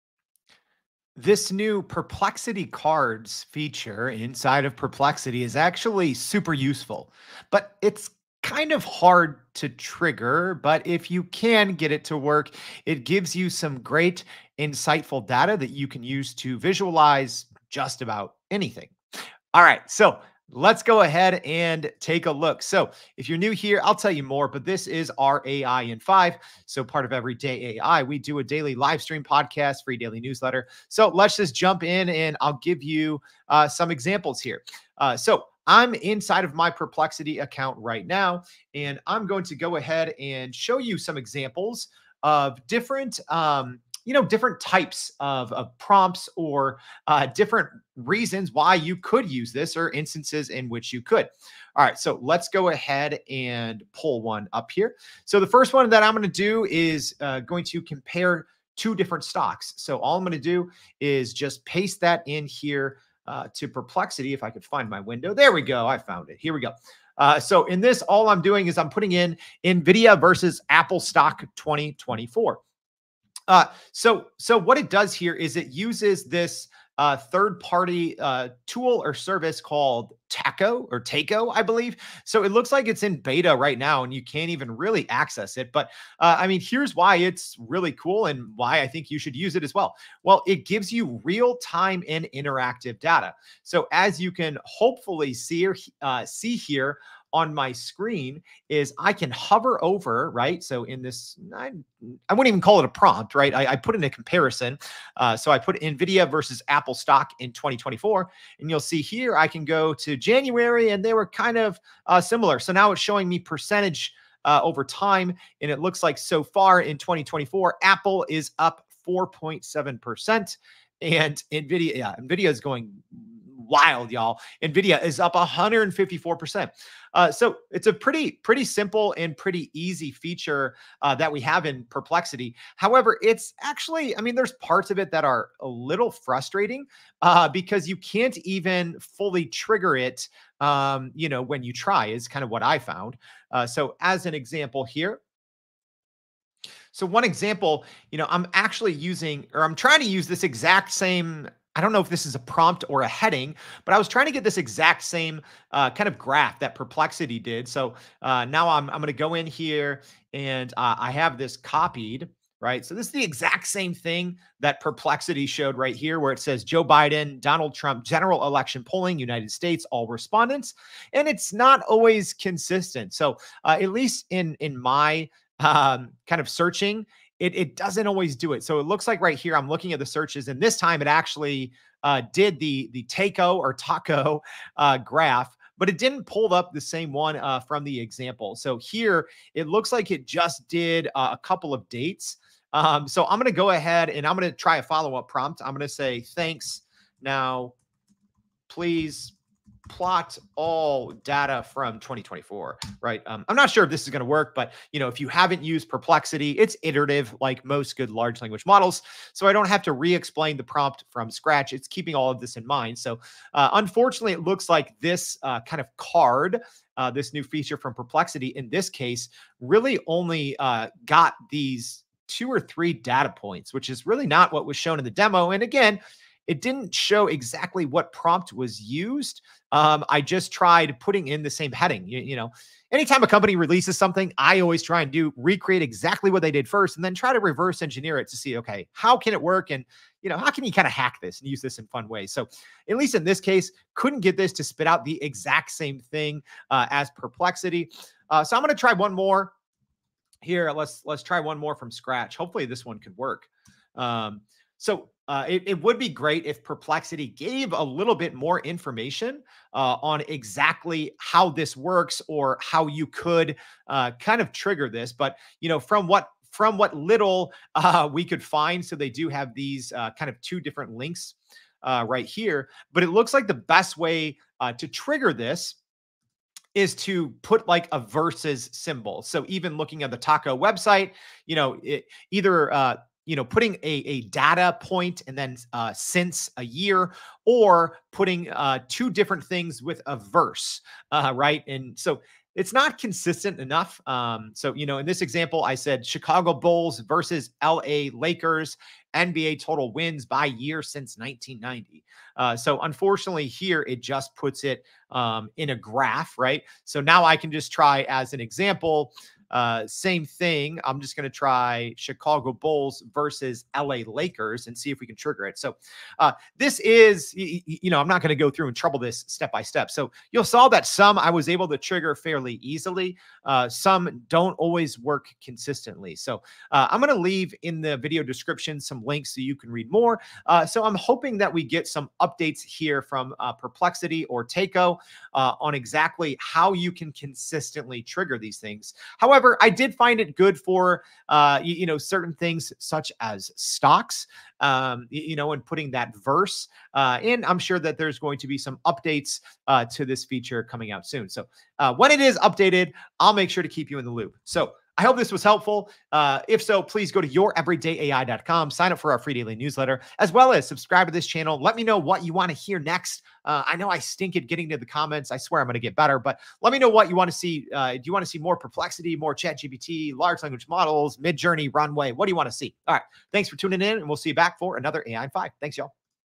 Perplexity Cards: How to (kinda) use them and how they help
In this video, we're diving deep into Perplexity Cards, showing you how to (kinda) use them and how they can help you visualize just about anything. Whether you're comparing stocks, polling data, or sports stats, these cards offer insightful data—though they might be a bit tricky to trigger consistently.

Curious about how to make this tool work for you? Want to see more features and updates? Drop a comment and let us know what you want to see next. Thanks for tuning in, and let's boost that productivity together! 🚀

CHAPTERS:
0:01 - Intro
0:36 - Comparing Stocks Analysis
2:24 - General Election Polling Insights
4:55 - NBA Wins by Year Trends
5:55 - Outro